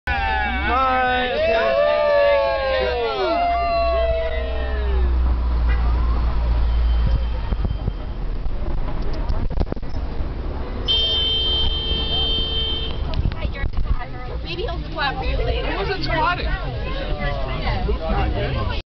Out the Hummer Sunroof
What is a party with a stretch hummer without going out the sunroof and acting like obnoxious bridge & tunnel trash?

Ed Oscar King
Oscar Party, March 5 2006

Eric Berkal